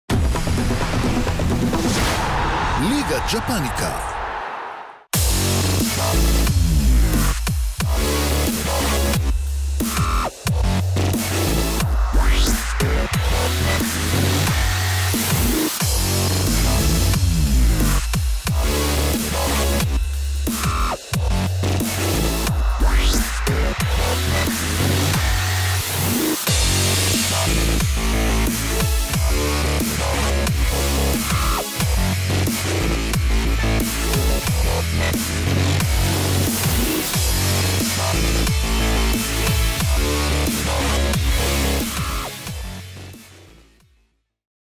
אנקדוטות לקראת המשחק בין מכבי ת"א למכבי נתניה
מוליכת הטבלה תנסה לשמור על מרחק מכל היתר, היהלומים יקוו להנחיל להם הפסד בכורה העונה. כל מה שכדאי לדעת לקראת המפגש בין שתיים מהקבוצות ההתקפיות ביותר בליגה